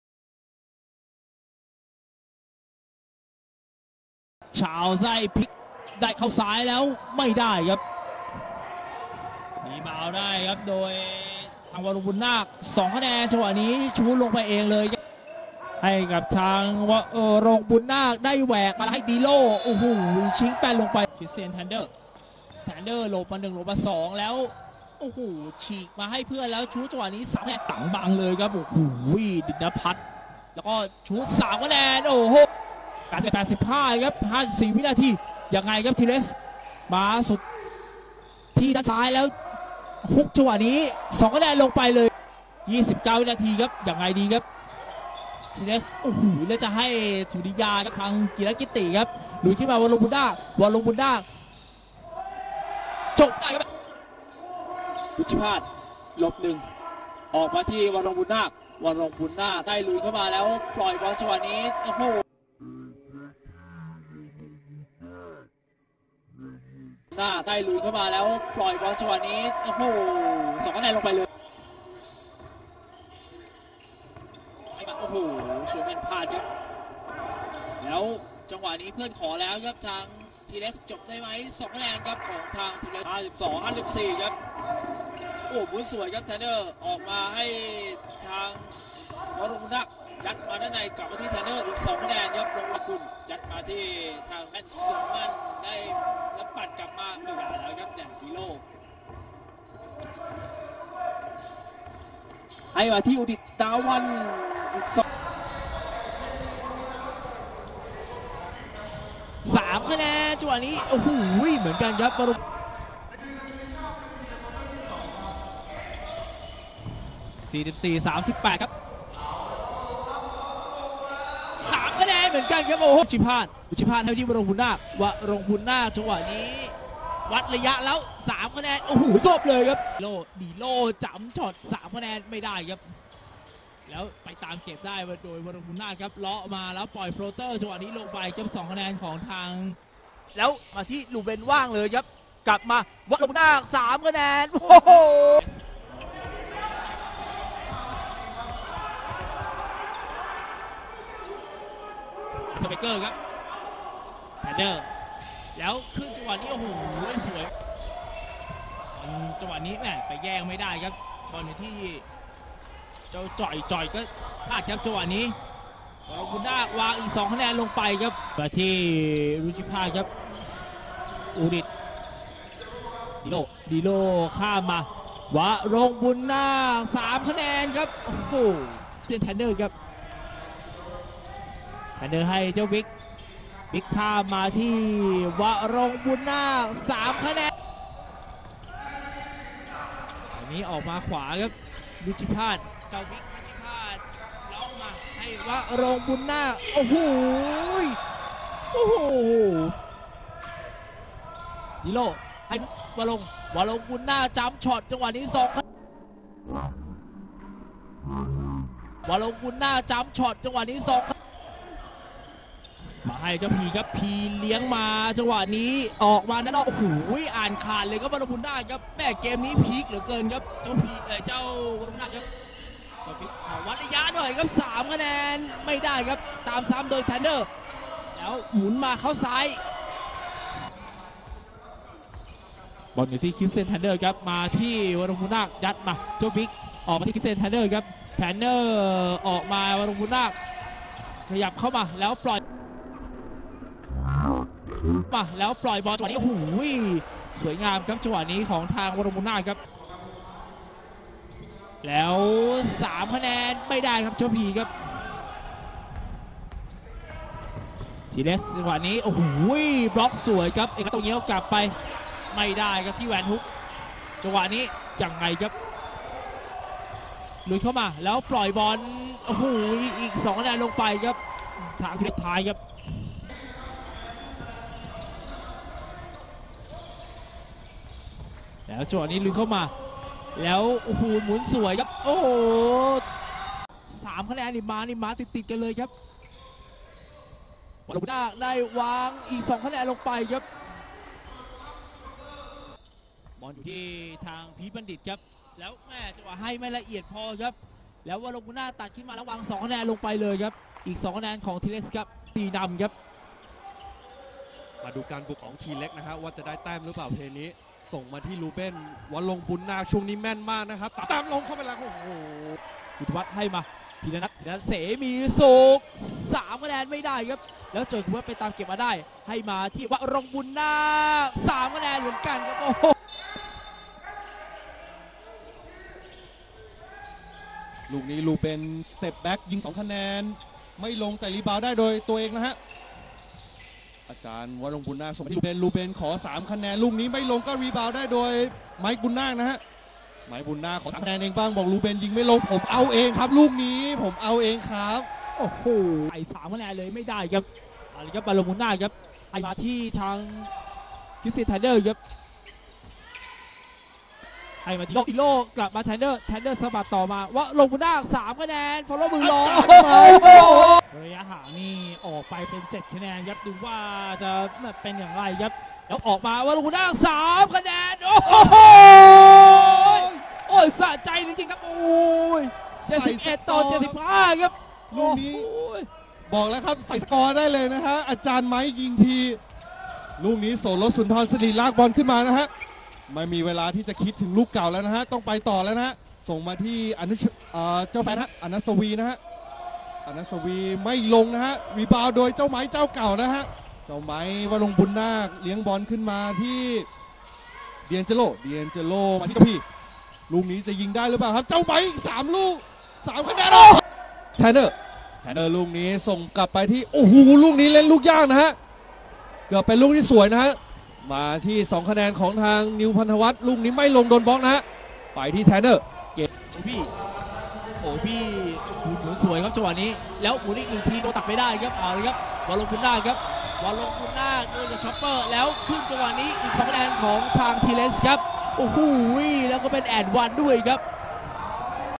2020 TBL Highlights - Warong (Mike) Boonnak - 1st Team TBL
2020 Stats - 12.5 P 7.6 R 3.4 A
Led team in steals

Background - 3 years in TBL/TBSL